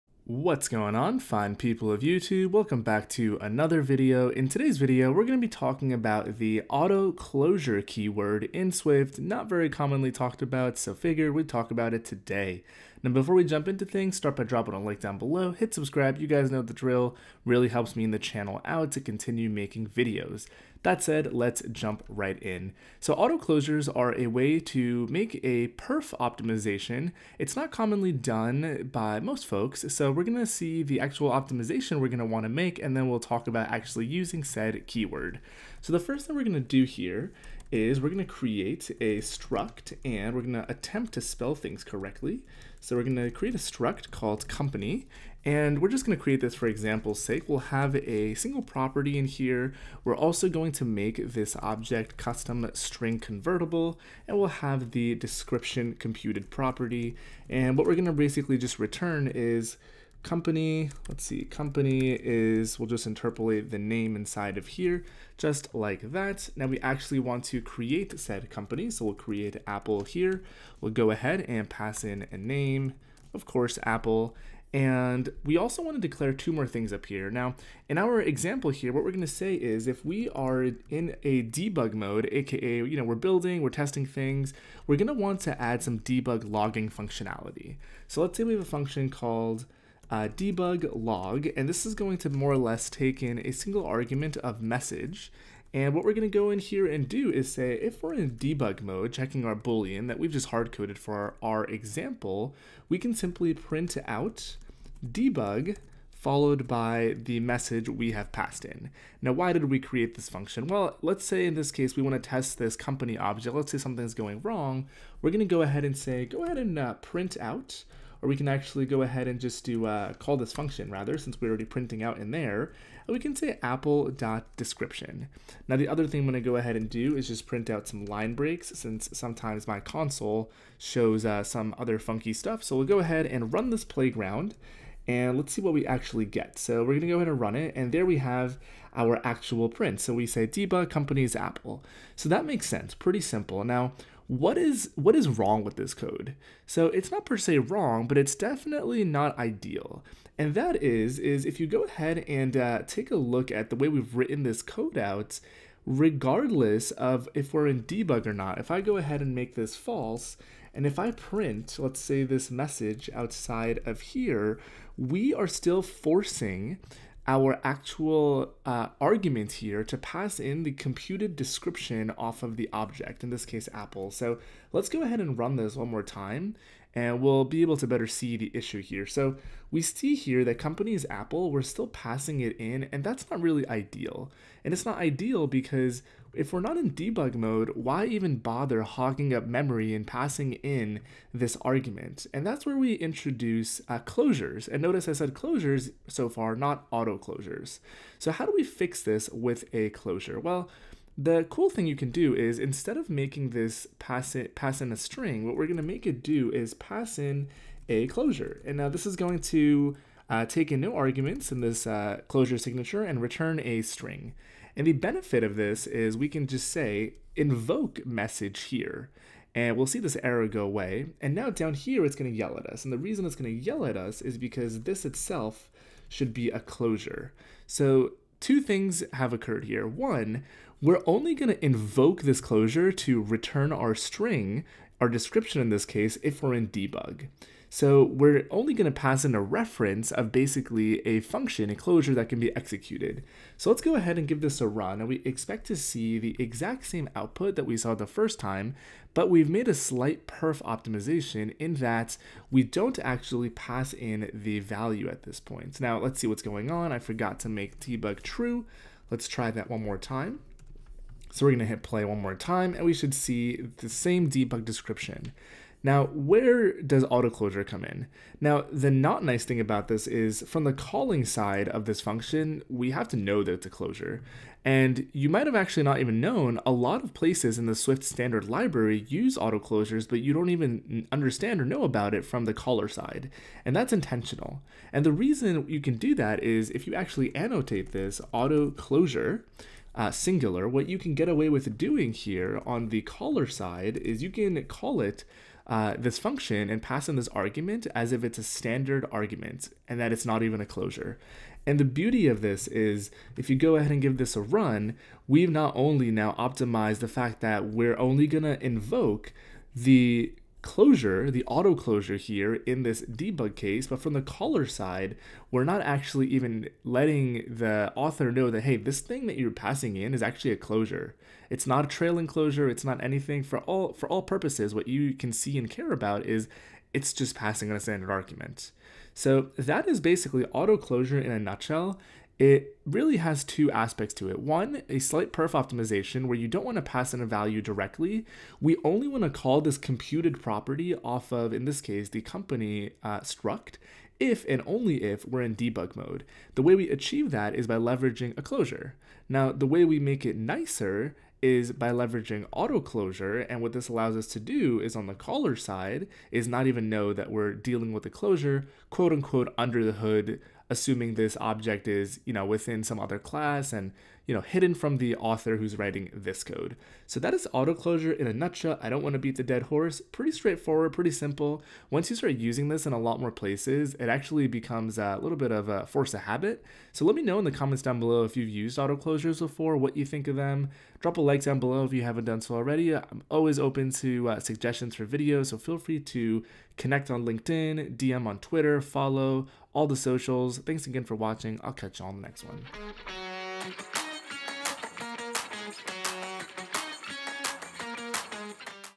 AutoClosures in Swift (Basics) – 2022
In todays video we will learn about the auto closure keyword in Swift, what its used for, and walk through a practical example. Used as a perf optimization, auto closures are designed to be useful but not obtrusive. Extensively used by the swift standard library, learn what benefit they provide. We’ll work in an Xcode playground with the latest version of Swift.